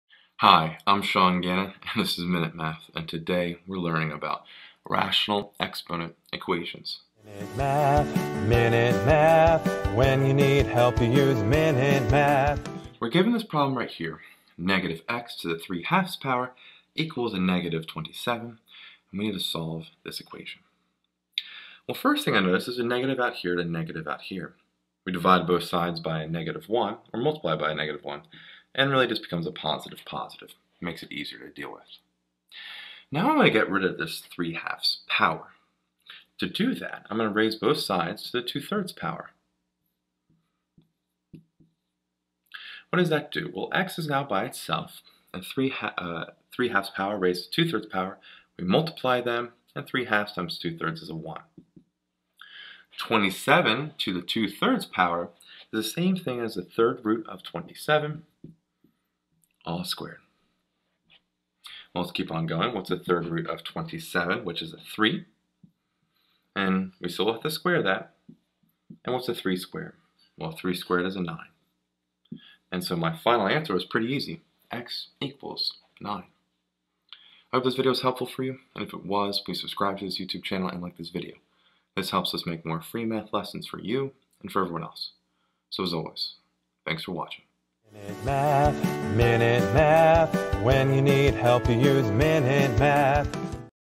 Solve -x^(3/2)=-27
Solving Rational Exponent Equations. We learn how to solve -x^(3/2)=-27. We learn how to solve the rational exponent equation -x^(3/2)=-27. This is a great introduction into rational exponents and how to solve rational exponent equations. This is under the topic of Radical Functions and Rational Exponents with this free online math video lesson.